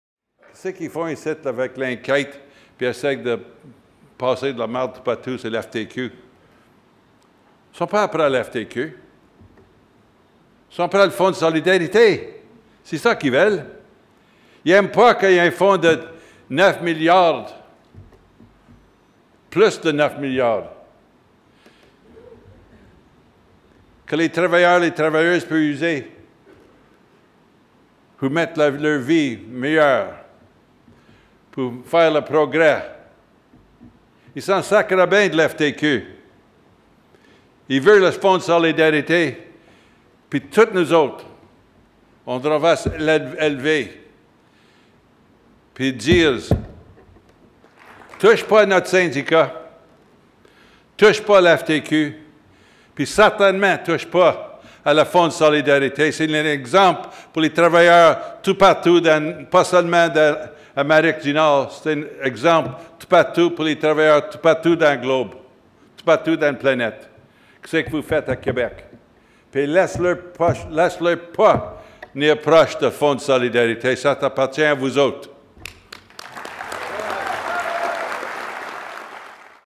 Leo W Gerard défend le Fonds de solidarité FTQ
Extrait de l'allocution de Leo W. Gerard lors de l'assemblée annuelle 2013 du Syndicat des Métallos au Québec. Il prend la défense du Fonds de solidarité FTQ, un outil essentiel pour les travailleurs et l'économie québécoise.